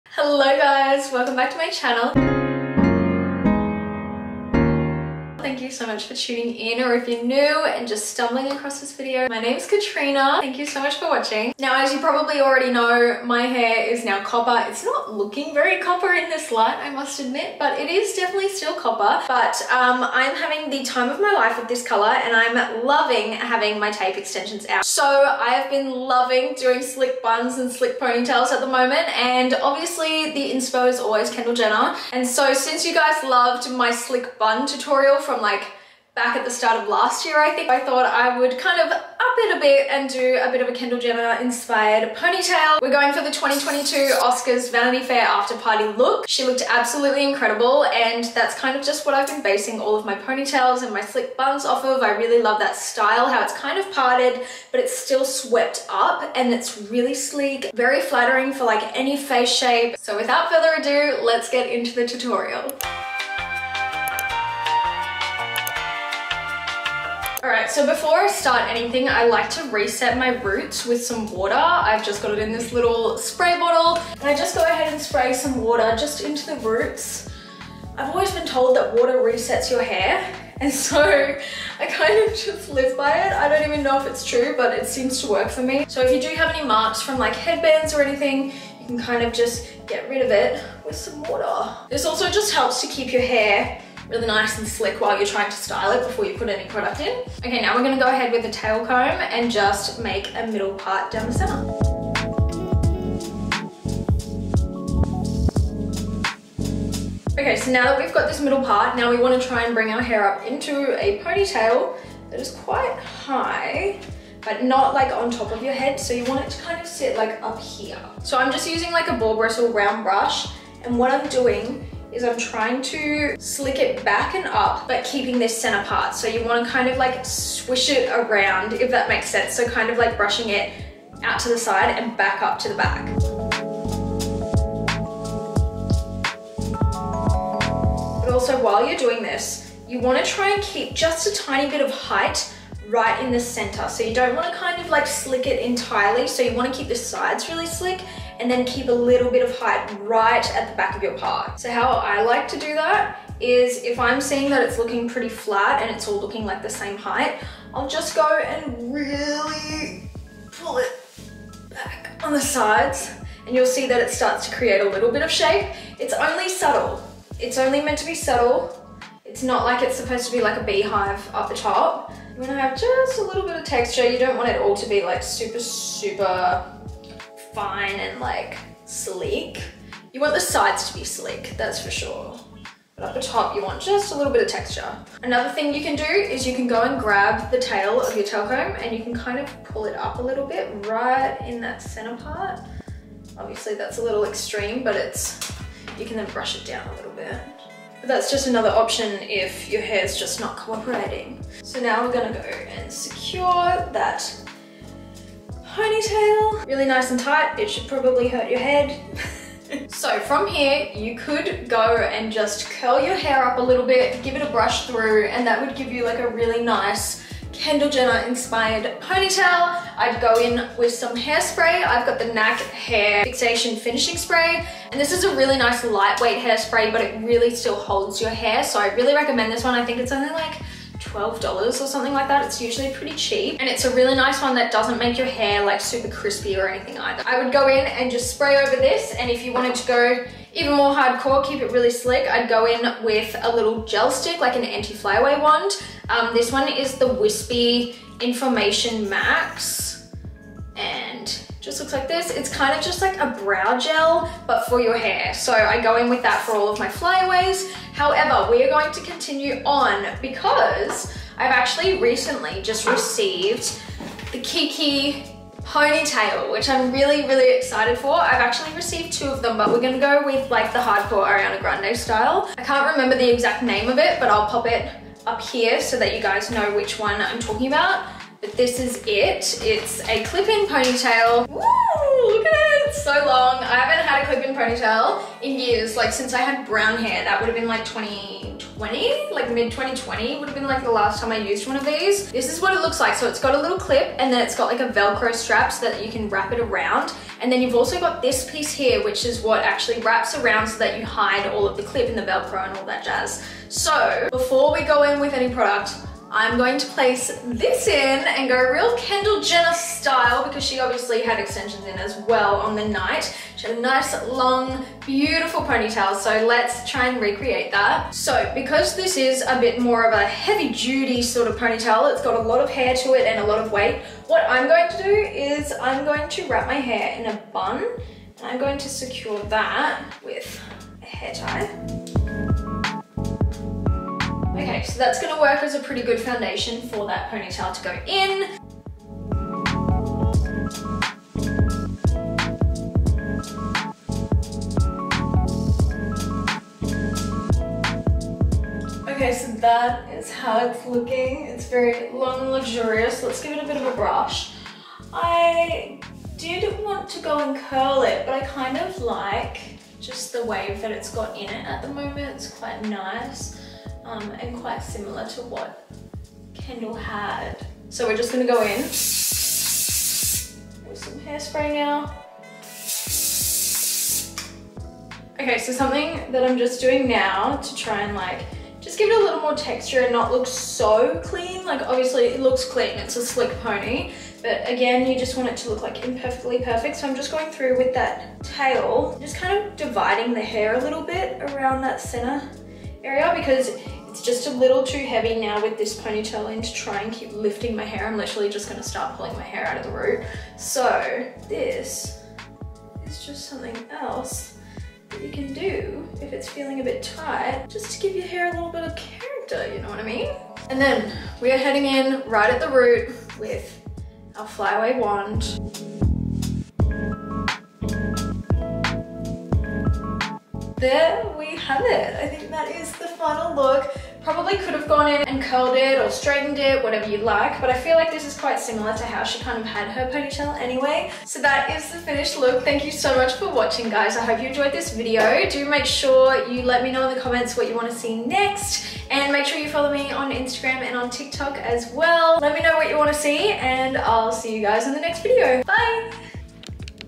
How To: Kendall Jenner Sleek Ponytail | Katrina West
I hope you guys enjoyed this Kendall Jenner inspired sleek ponytail hairstyle tutorial!